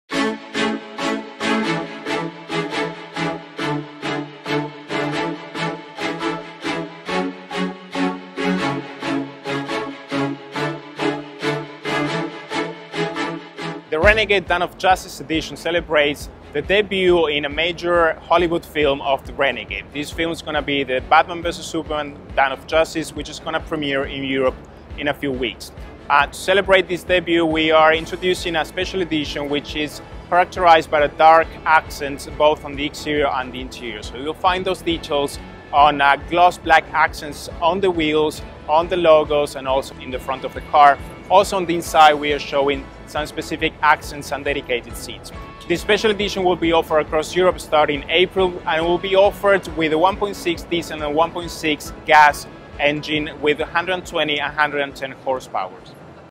Intervista a Dante Zilli - Jeep Renegade
Dopo il successo dell’anteprima mondiale al Salone di Ginevra, la nuova Jeep Renegade Dawn of Justice Special Edition è già disponibile presso i concessionari italiani.
Prodotta in tiratura limitata, questo nuovo modello presenta una caratterizzazione estetica particolarmente distintiva grazie ai tanti elementi, esterni e interni, verniciati in nero lucido quali i cerchi in lega da 18", le cornici dei fendinebbia e dei fari posteriori, la griglia frontale e il logo Jeep, oltre a numerosi dettagli di stile presenti nell’abitacolo. Il risultato finale è un effetto estetico di grande impatto e di assoluta coerenza con il posizionamento del modello.